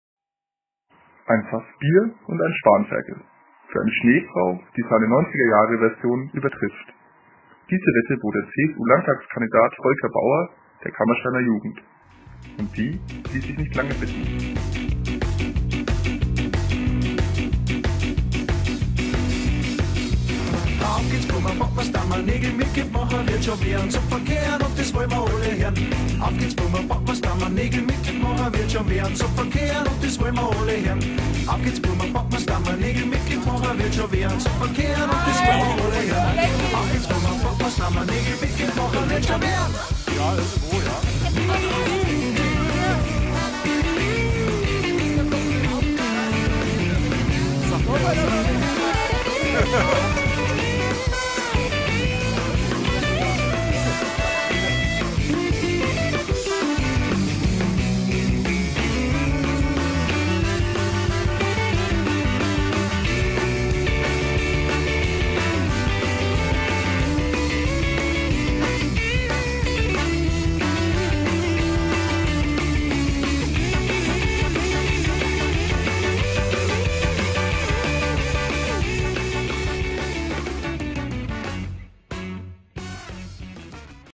Ramunga - Bayerns größte Schneefrau initiiert durch Volker Bauer (Musik: ZiehGäuner)
Der größte Schneemann Deutschlands steht in Oberfranken -- aber die wohl größte Schneefrau Bayerns steht im Kammersteiner Ortsteil Haag. Florian Sander und Helfer der Freiwilligen Feuerwehr Kammerstein und den Haager Maifest-Freunden hatten eine Wette des Urgesteins der Kammersteiner Jugendarbeit Volker Bauer angenommen: der heutige CSU-Landtagskandidat hatte vor 18 Jahren mit der ELJ eine 3,80 Meter hohe Schneefrau gebaut. Diese Höhe musste überboten werden.

Vor 18 Jahren hatte Bauer selbst mit der ELJ eine knapp 3,80 Meter hohe Schneefrau gebaut und nun mit der Jugend seiner Heimatgemeinde Kammerstein gewettet, dass sie es nicht schaffe, diese Höhe zu überbieten. Nach zweitägiger Bauzeit können Anwohner und Autofahrer direkt neben der B466 in Haag die fünf Meter hohe „Ramunga", Bayerns größte nur aus Schnee bestehende Frau, bewundern und -- hoffentlich kein Hinweis auf das Geschlechterverhältnis in Kammerstein -- ihren zwei Meter „kleinen" Mann.